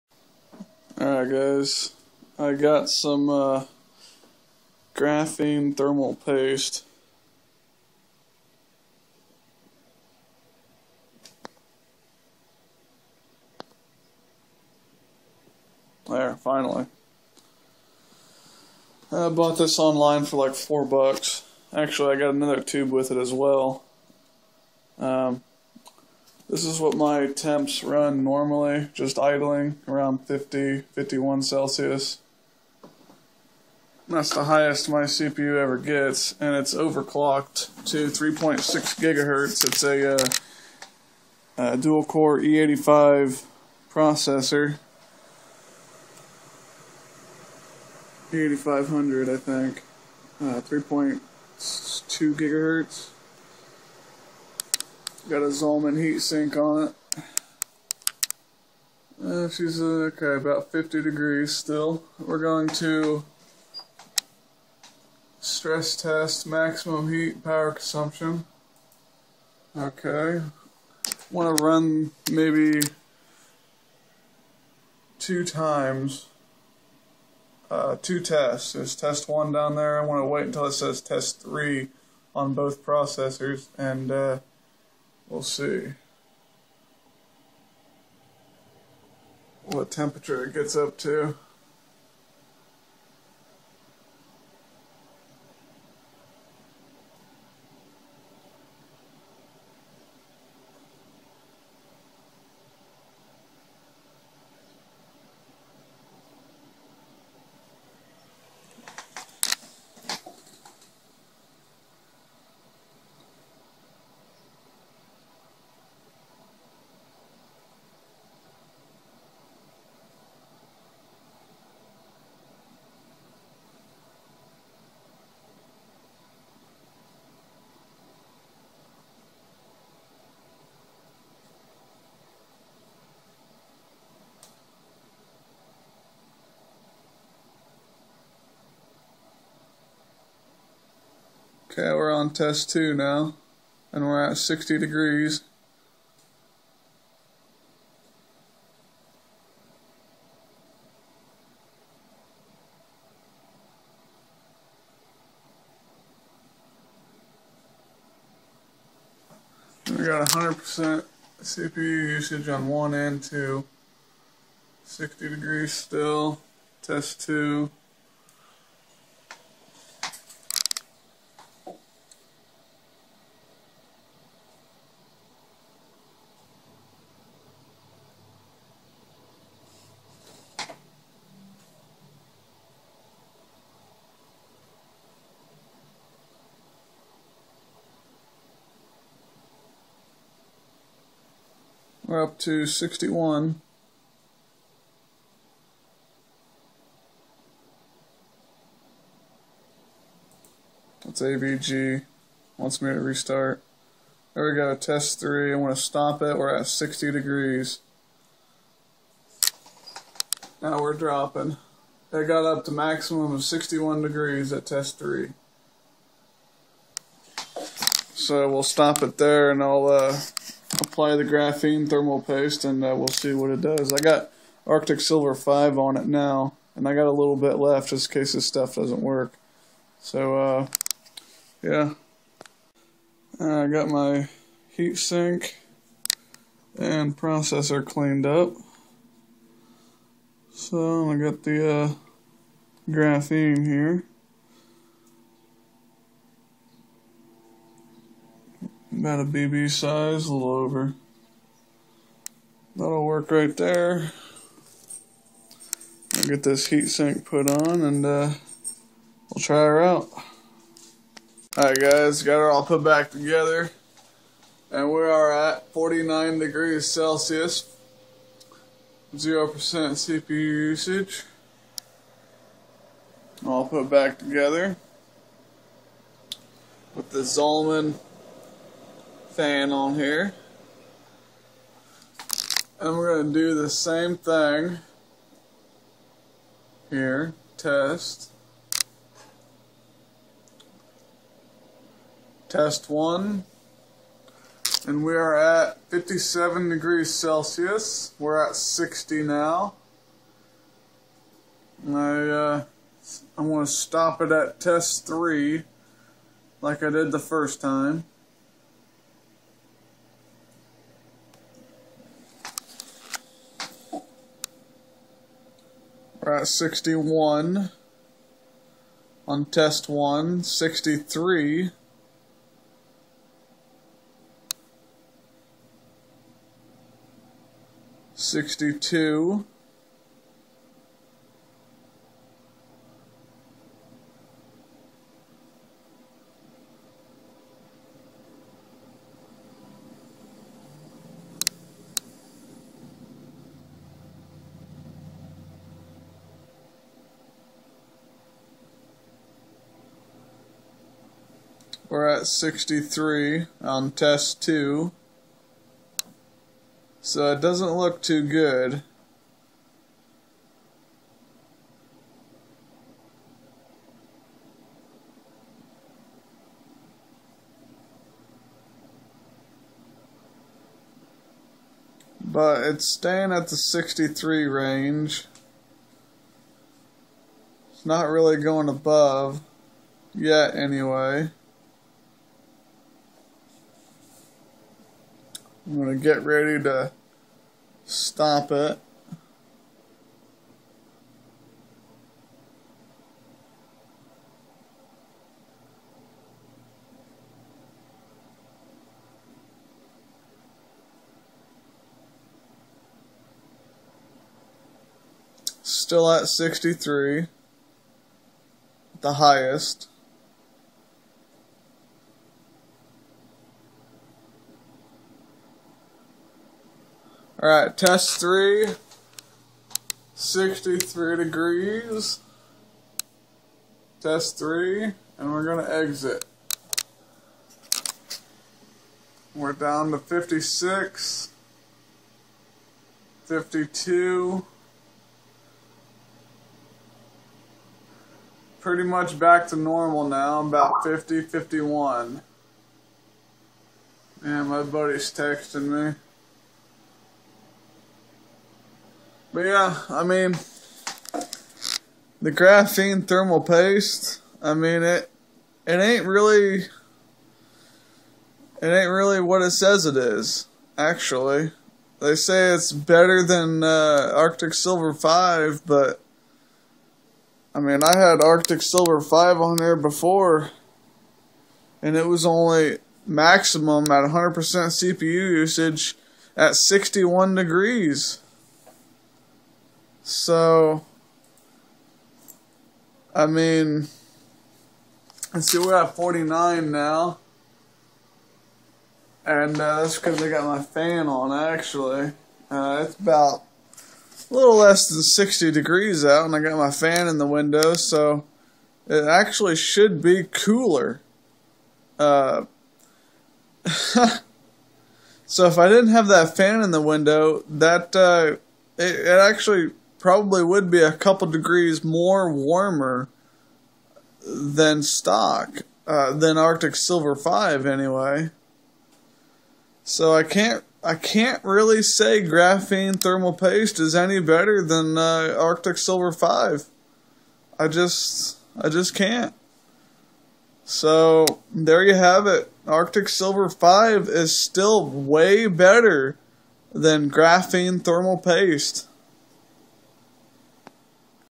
Graphene vs Arctic silver 5 thermal paste
in this video I test out the grapheme thermal paste against arctic silver 5 thermal paste